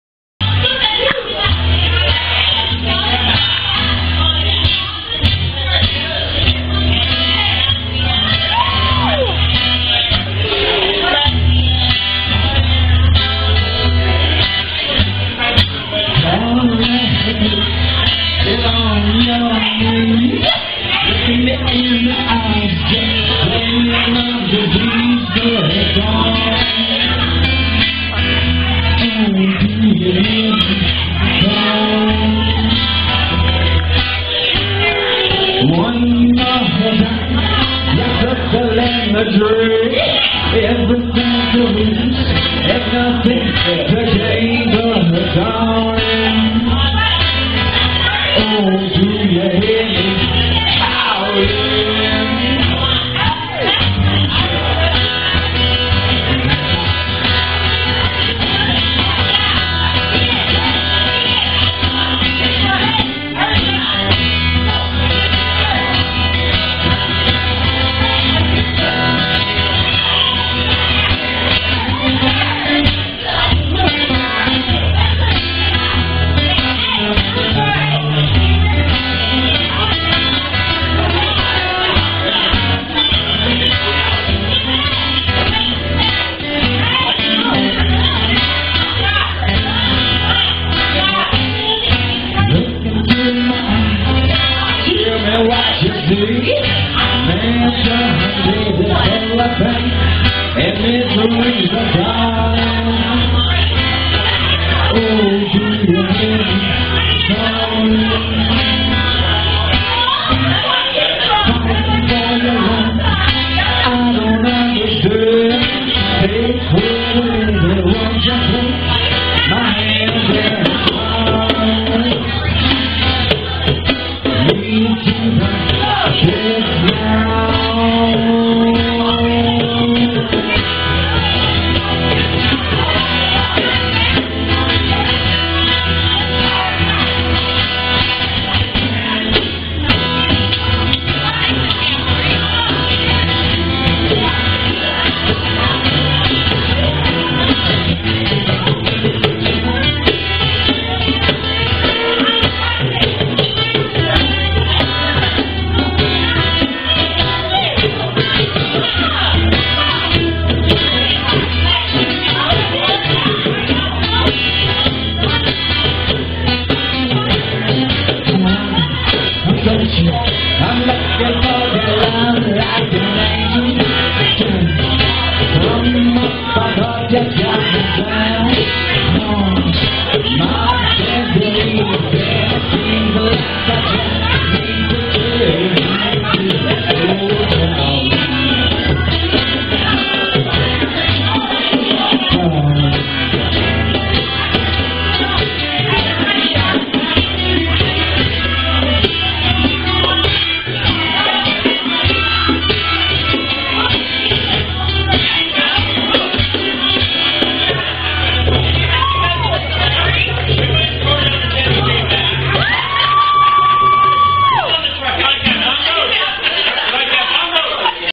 SRC & DK Ben Boren 2
Steven Christie playing at The Bighorn Saloon...BUT letting Ben Boren get up and play a few originals and DK with the assist.